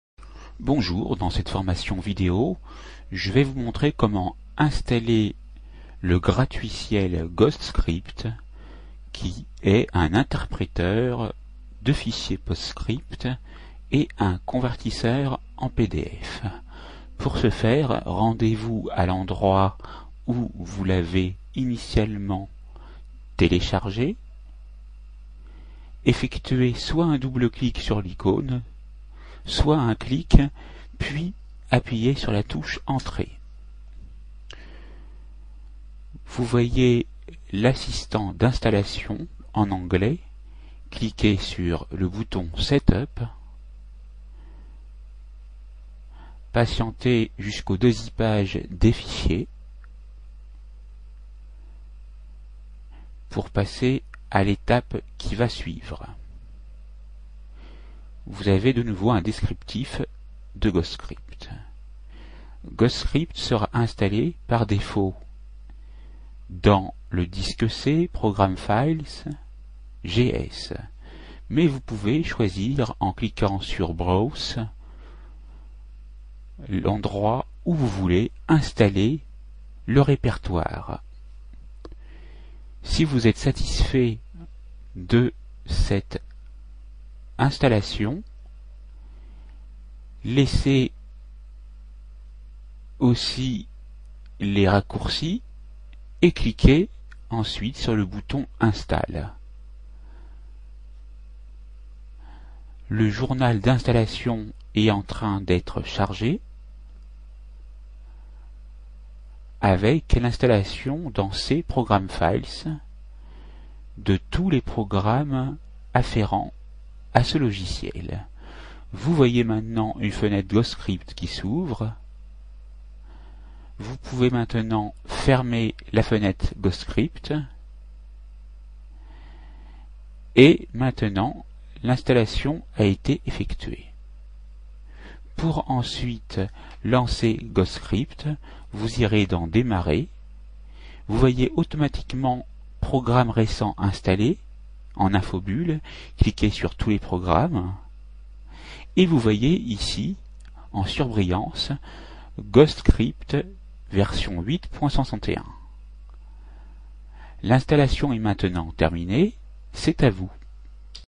Installer le gratuitiel Ghostscript
Dans ce tutoriel vidéo d'informatique, je vais vous montrer comment installer le gratuitiel Ghostscript. Ghostscript est un logiciel gratuit en langue anglaise qui se compose d'un panel de logiciels ayant pour mission d'analyser, de traduire en divers autres formats mais aussi d'exécuter les programmes écrits en PostScript et PDF, de les visualiser et de les imprimer. Les fichiers PostScript et PDF pourront même être réciproquement traduits d'un langage vers l'autre. Ghostscript intègre des fonctionnalités capables d'améliorer les graphismes habituellement réduits au strict minimum sur les fichiers aux formats PostScript et PDF.